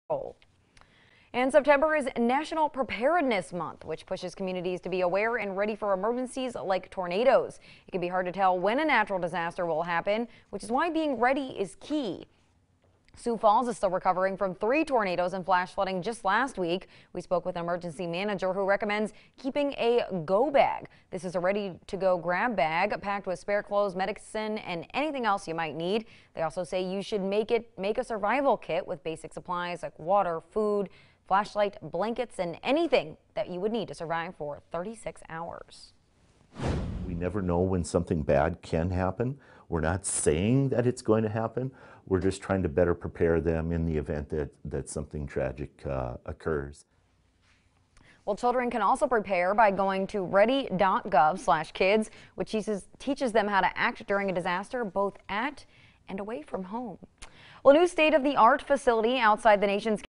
Emergency Preparedness
A North Dakota Emergency Manager talks prepping for any emergency